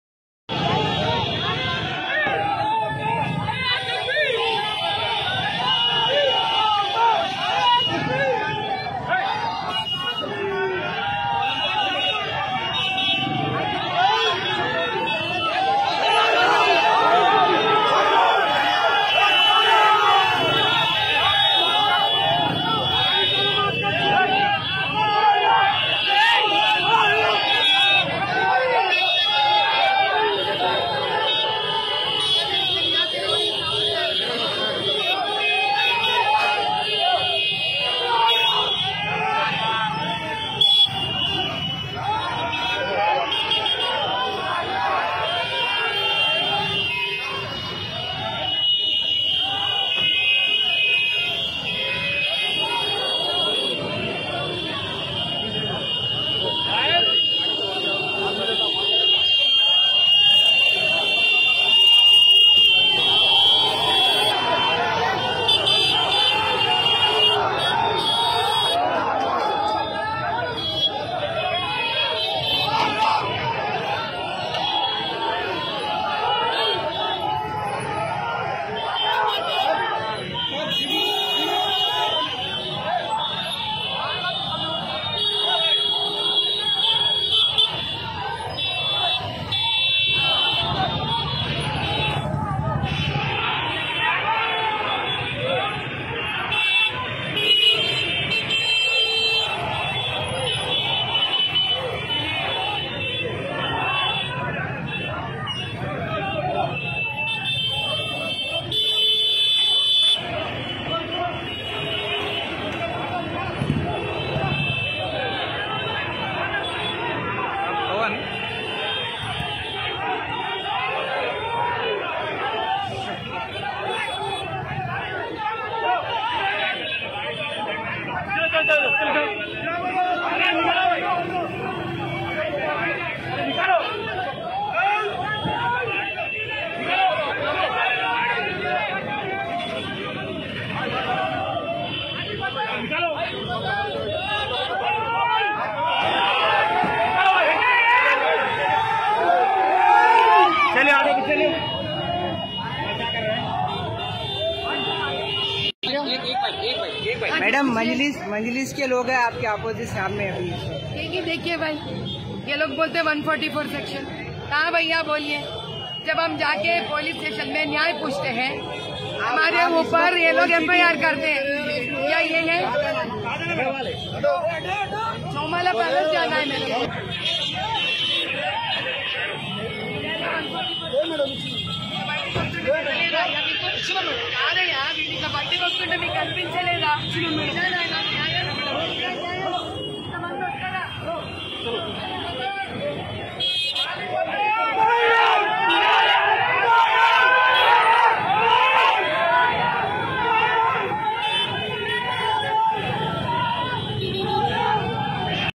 Hyderabad news Bjp madhavi latha and asaduddin uddin owasi
चुनाव के बीच में बीजेपी के उम्मीदवार माधवी लता के ऊपर केस डालने को लेकर लोगों ने किया बवालमाधवी लता ने भी कहा कि मेरे साथ यह. छुटपुट घटनाओं के साथ हैदराबाद में चुनाव संपन्न